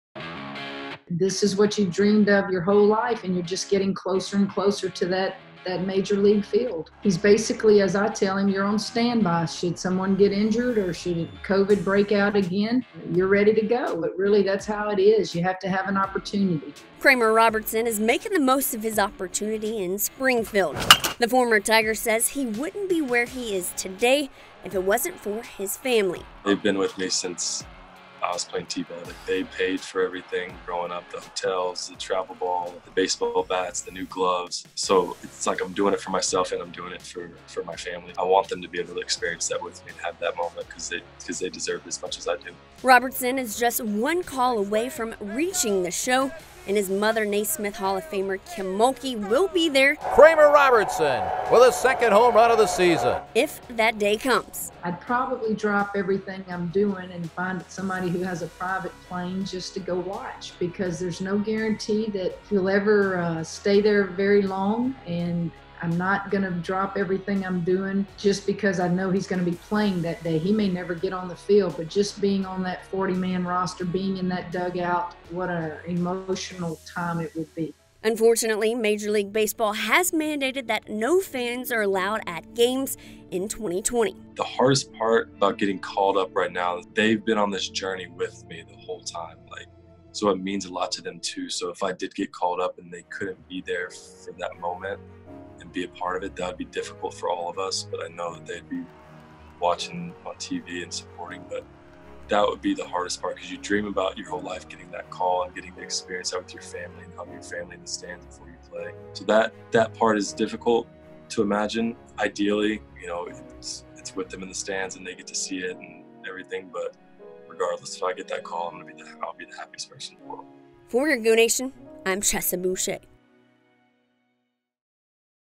Robertson eager for his shot in the "Show"
Former LSU All-American Kramer Robertson is making the most of his time in Springfield with the St. Louis Cardinals taxi squad.